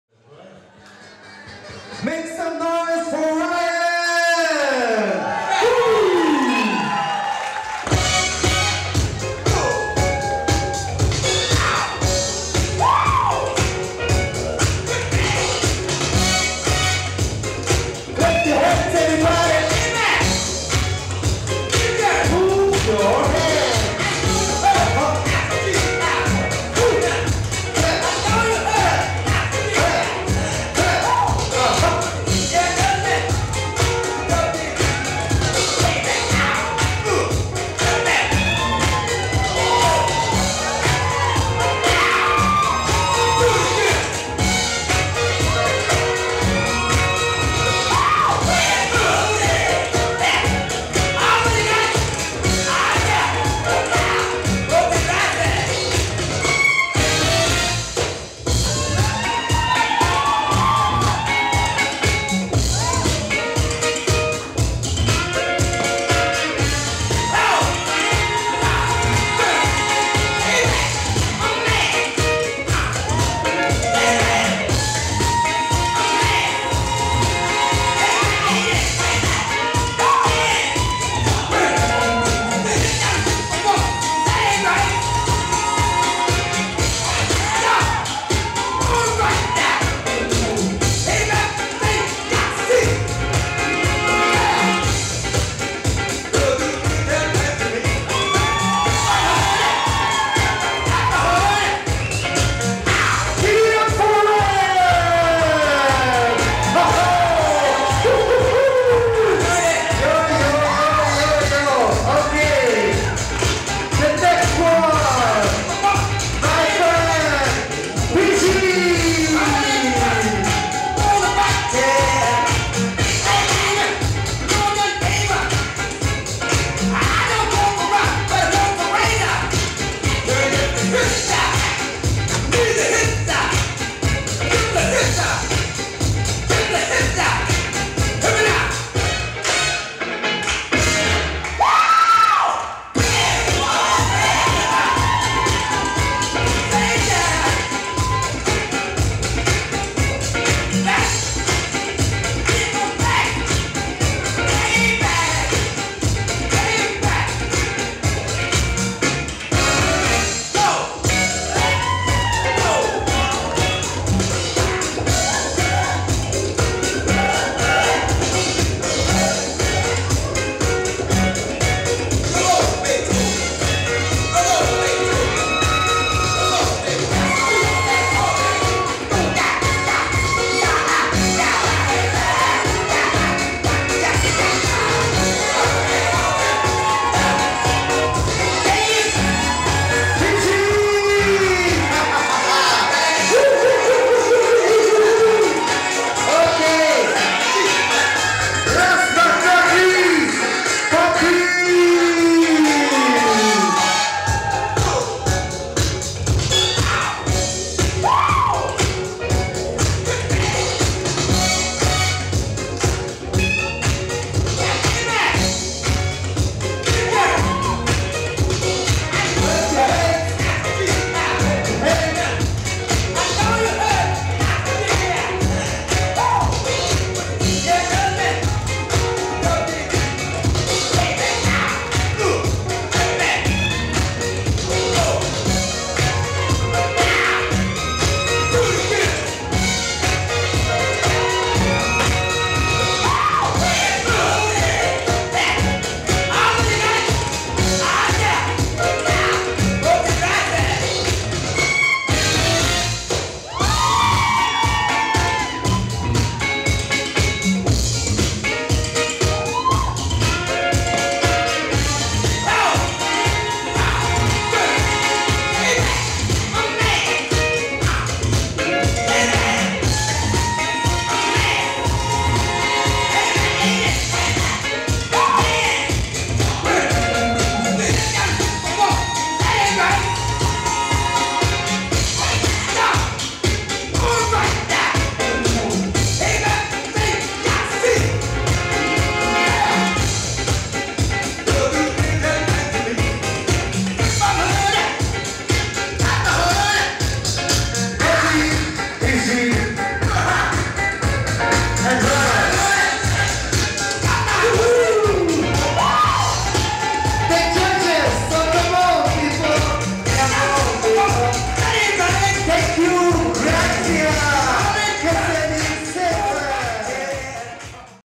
LOCKING JUDGES - RYAN, PIZSY, PAPI - UD KEMP 2012
5th Urban Dance Kemp - international hiphop dance camp - Balinka, Hungary - 15-22. july 2012.